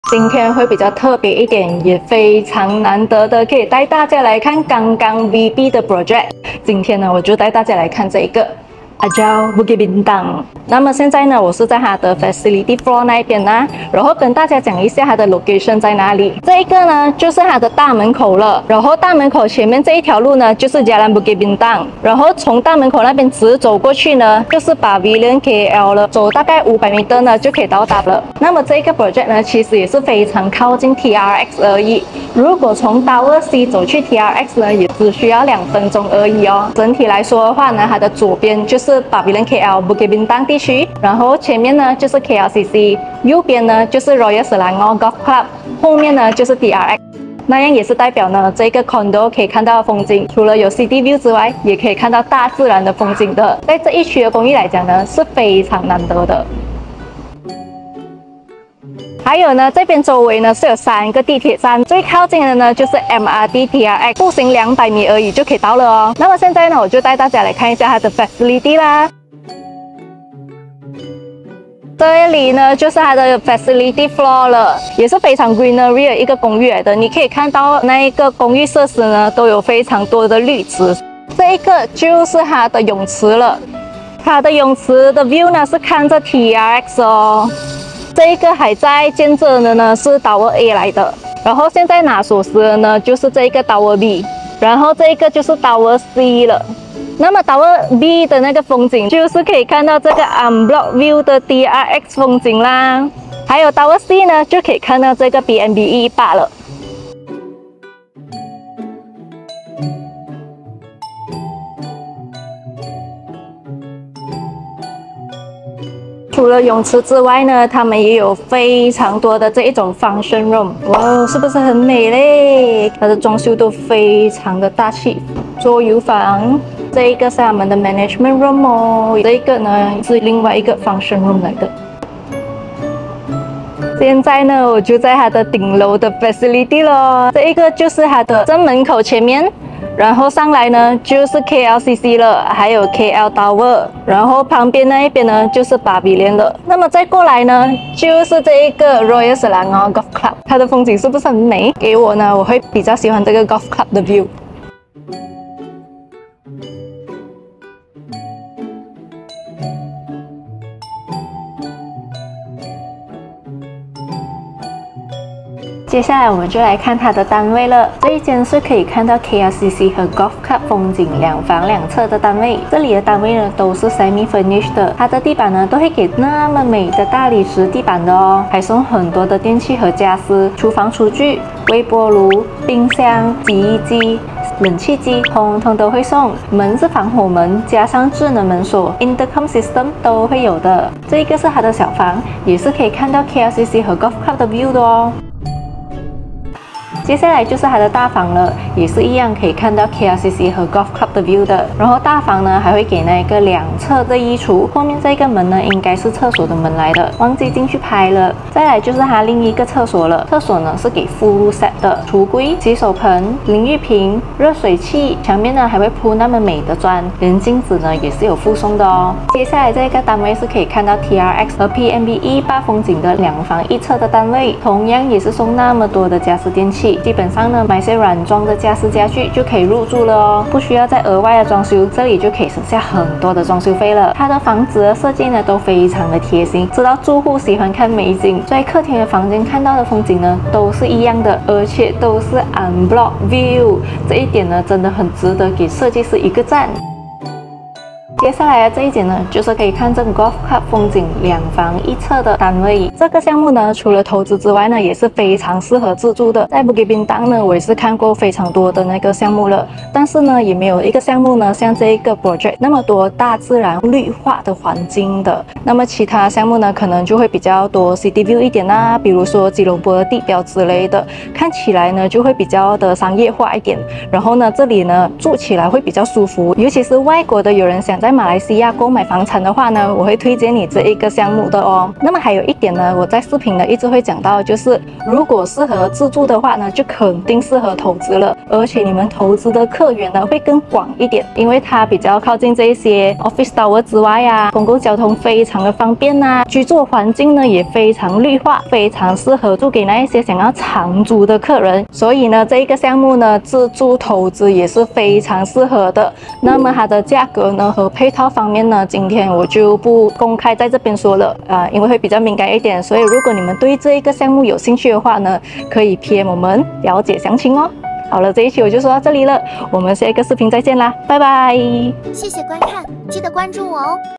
𝐑𝐞𝐯𝐢𝐞𝐰 #𝟏𝟏 - 𝐕𝐏拿锁匙𝐀𝐠𝐢𝐥𝐞 𝐁𝐮𝐤𝐢𝐭 𝐁𝐢𝐧𝐭𝐚𝐧𝐠 ✅走路2分钟到𝐓𝐑𝐗 ✅𝟓𝟎𝟎𝐦到𝐏𝐚𝐯𝐢𝐥𝐢𝐨𝐧 ✅刚刚才拿锁匙
🌟 地点就在𝐓𝐑𝐗的后面
📍 步行𝟐分钟到𝐓𝐑𝐗
📍 𝟓𝟎𝟎𝐦到𝐏𝐚𝐯𝐢𝐥𝐢𝐨𝐧 𝐊𝐋

✅ 在𝐓𝐑𝐗和𝐏𝐚𝐯𝐢𝐥𝐢𝐨𝐧 𝐊𝐋的中心点
✅ 附近有三个走路都可以到的地铁站
✅ 刚刚才拿锁匙

很开心有机会来探刚刚拿锁匙的公寓

泳池可以看到一整个TRX就在眼前
真的是太惊艳了
还有顶楼设施可以欣赏美景

公寓的客厅和房间可以看到吉隆坡无遮挡的风景
🌟𝐓𝐑𝐗
🌟𝐏𝐍𝐁𝟏𝟏𝟖
🌟𝐊𝐋𝐂𝐂
🌟𝐑𝐨𝐲𝐚𝐥 𝐒𝐞𝐥𝐚𝐧𝐠𝐨𝐫 𝐆𝐨𝐥𝐟 𝐂𝐥𝐮𝐛皇家高尔夫球场
✨ 都是𝐮𝐧𝐛𝐥𝐨𝐜𝐤 𝐯𝐢𝐞𝐰，整大片风景就在眼前
✨ 想看城市高楼还是大自然的美景都由你选择

🌟 公寓刚刚安排拿锁匙
✅ 不需要担心烂尾/货不对板，直接看𝐚𝐜𝐭𝐮𝐚𝐥 𝐮𝐧𝐢𝐭
✅ 省掉建筑利息
✅ 所有东西都是新的，拿了锁匙就可以立刻入住/出租

✨公寓还会附送很多家私电器
✨又省掉一大笔的装修费了～

👇🏻任何的优惠配套和价格咨询请PM👇🏻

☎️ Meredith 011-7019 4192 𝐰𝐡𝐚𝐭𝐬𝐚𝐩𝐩